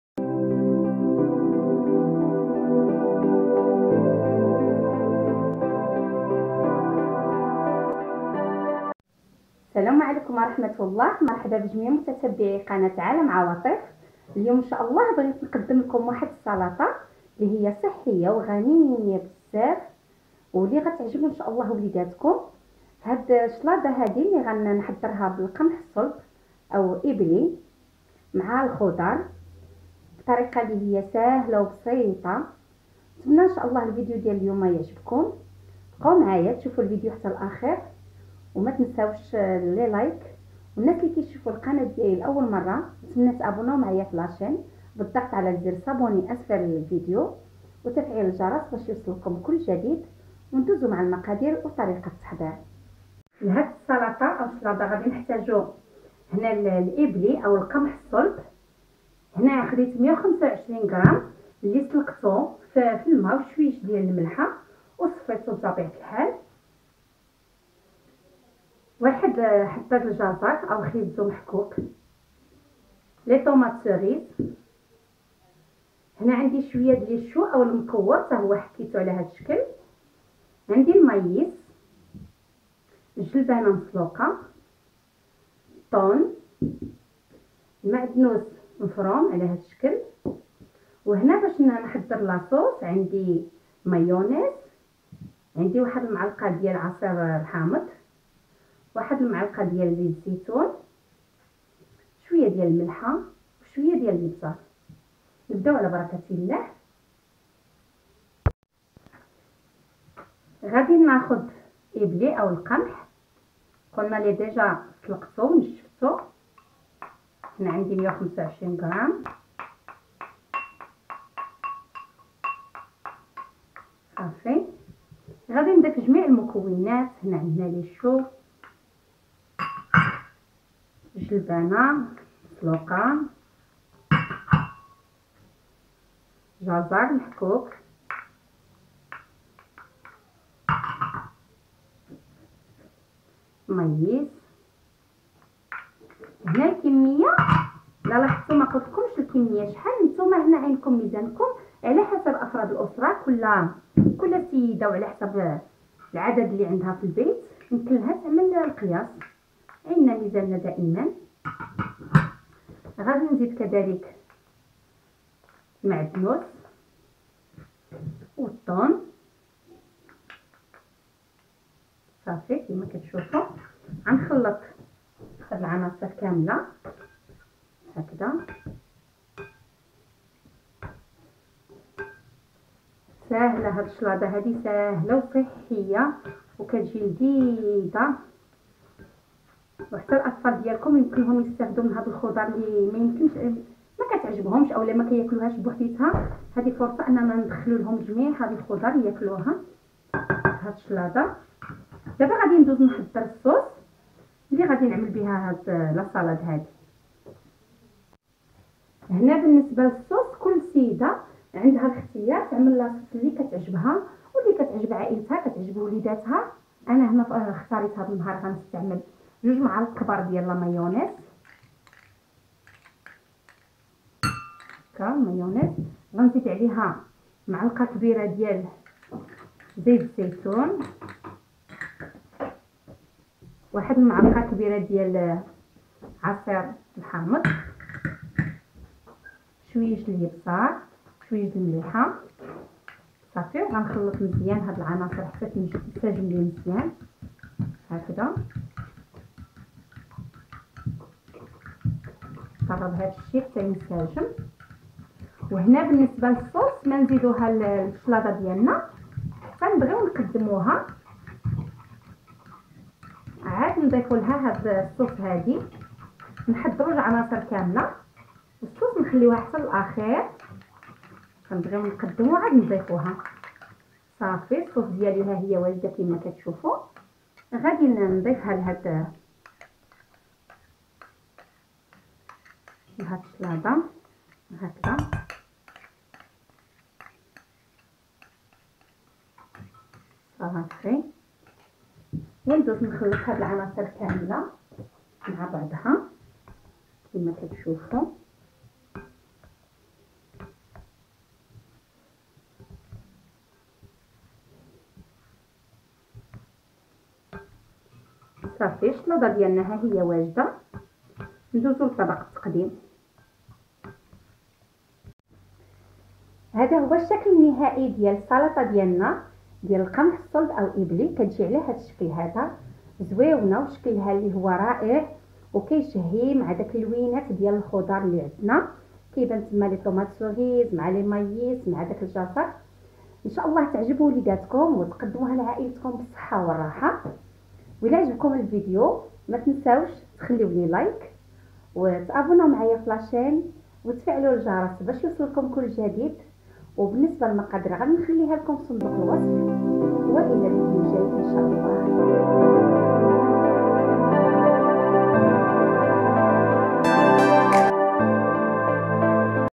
سلطة بالقمح ايبلي طون و الخضر غنية جدا و شهية
عالم عواطف تقدم لكم سلطة بالقمح  ايبلي  طون و الخضر غنية جدا و شهية
قناتنا هذه تقدم لكم وصفات للطبخ المغربي وغيره ، حلويات،معجنات, أكلات سهلة التحضير بتكلفة قليلة .نرجوا أن تنال إعجابكم

المقادير :
125غ قمح
حبة جزر
tomate cerise
ذرة mais
جلبانة
الكرنب
طون
معدنوس

لتحضير الصلصة
2معالق كبار مايونيز
ملعقة كبيرة زيت الزيتون
ملعقة كبيرة عصير الحامض
ابزار و ملح

وصفاتي السابقة